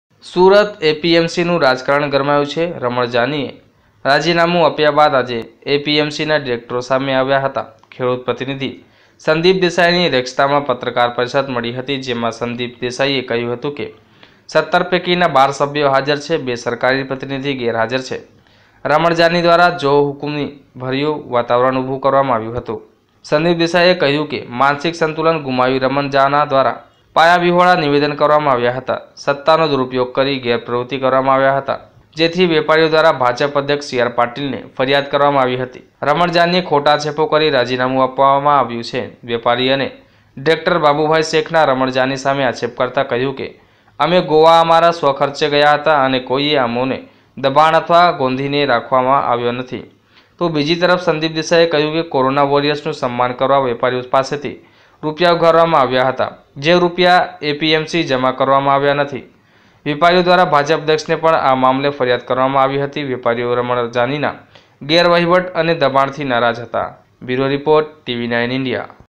સુરત યાર્ડના ડિરેક્ટરોએ કહ્યું-'રમણ જાની દ્વારા જોહુકમી ભર્યુ વાતાવરણ ઉભું કરાયું.|TV9INDIA GUJARATI
સુરત એપીએમસીનું રાજકારણ ગરમાયું છે. રમણ જાનીએ રાજીનામું આપ્યા બાદ આજે એપીએમસીના ડિરેક્ટરો સામે આવ્યાં હતાં. ખેડૂત પ્રતિનિધિ સંદીપ દેસાઈની અધ્યક્ષતામાં પત્રકાર પરિષદ મળી હતી. જેમાં સંદીપ દેસાઈએ કહ્યું હતું કે, 17 પૈકીના 12 સભ્યો હાજર છે. બે સરકારી પ્રતિનિધિ ગેરહાજર છે.રમણ જાની દ્વારા જોહુકમી ભર્યું વાતાવરણ ઉભું કરવામાં આવ્યું હતું.